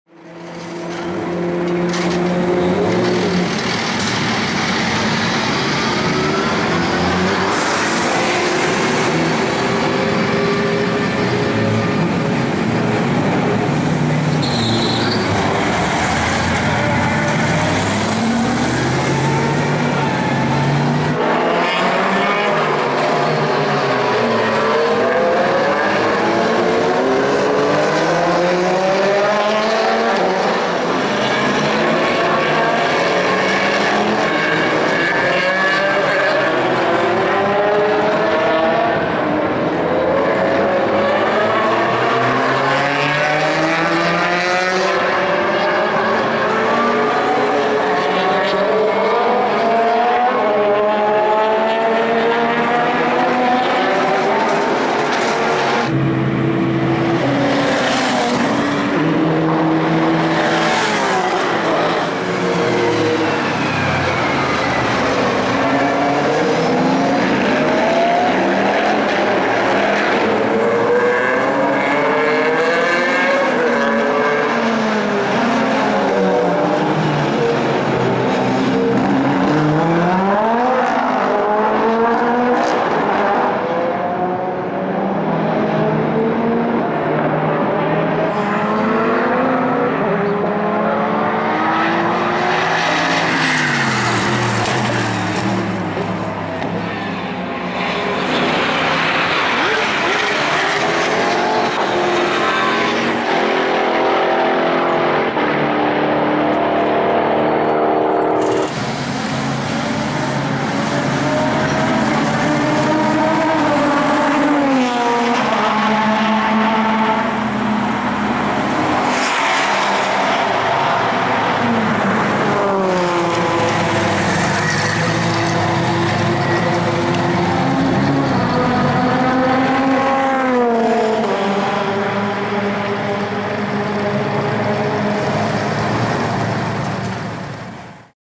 dress-for-less Racing Formula Le Mans Nürburgring 2012
Here is my Video from the dress-for-less Racing Formula Le Mans with Mirco Schulties at Nürburgring 2012. The Car won the Formula Le Mans Class at Le Mans Series 2011.

RF 1993 Sportscars Racing Videos